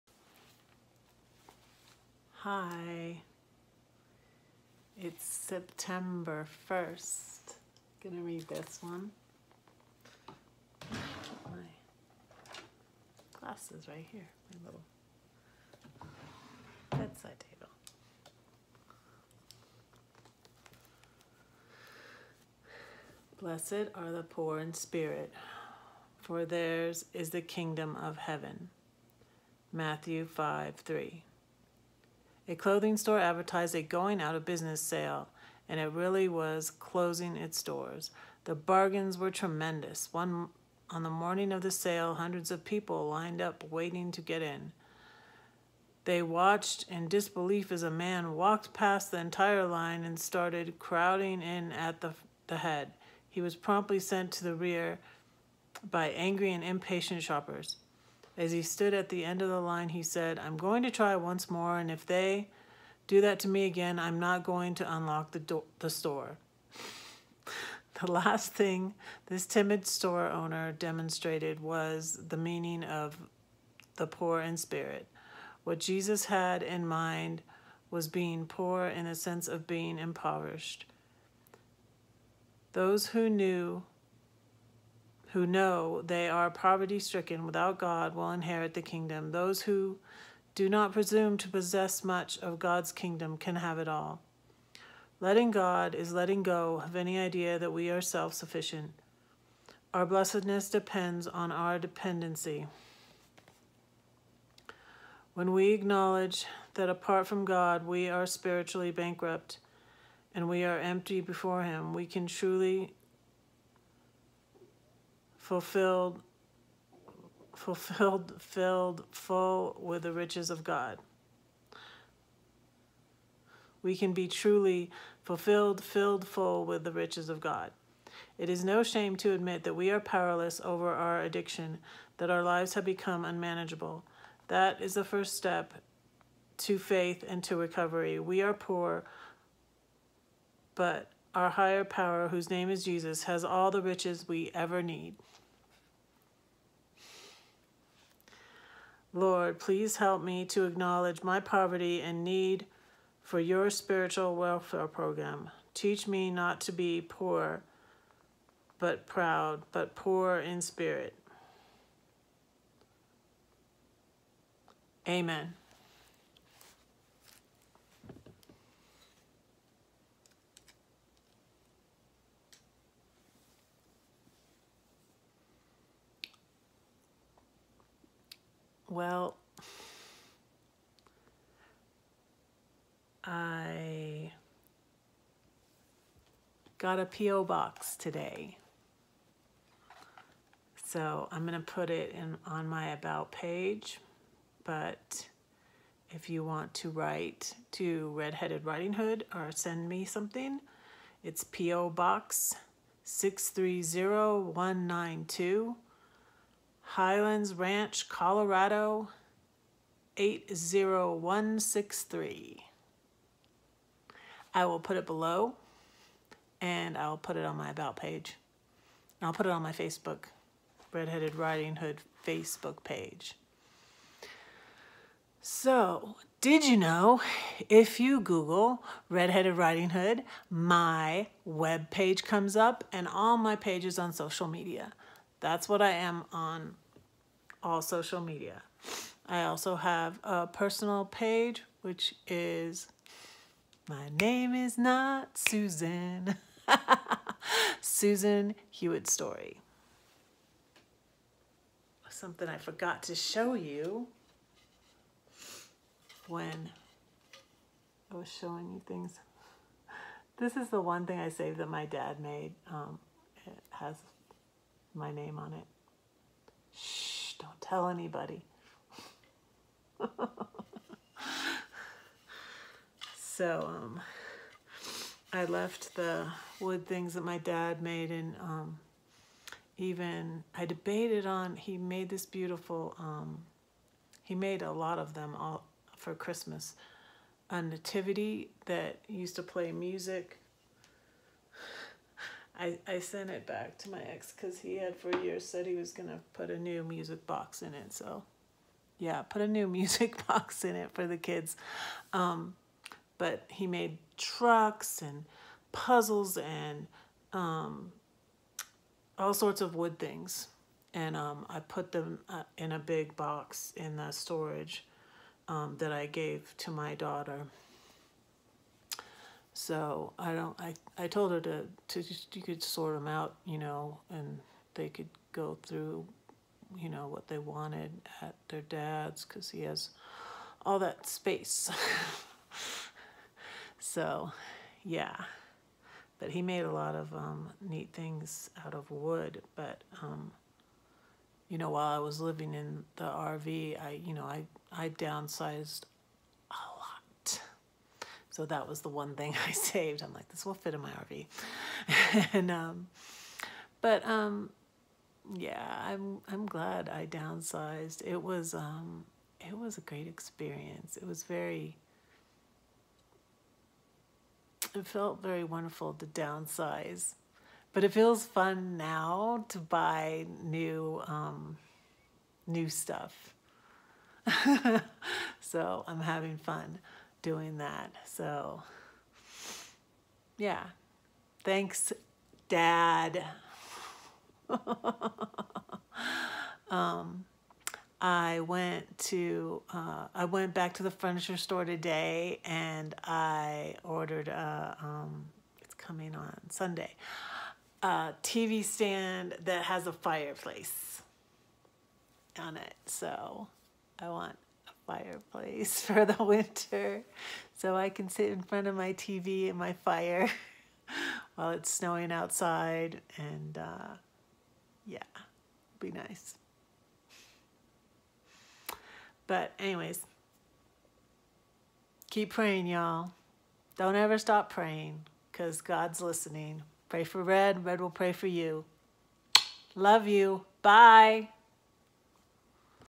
Devotion With Red: Blessed Are The Poor In Spirit
This devotion is from, "Letting God: Christian Meditations For Recovering Persons" by A. Philip Parham
You may write to me at:

Redheaded Writing Hood
Highlands Ranch, CO
80163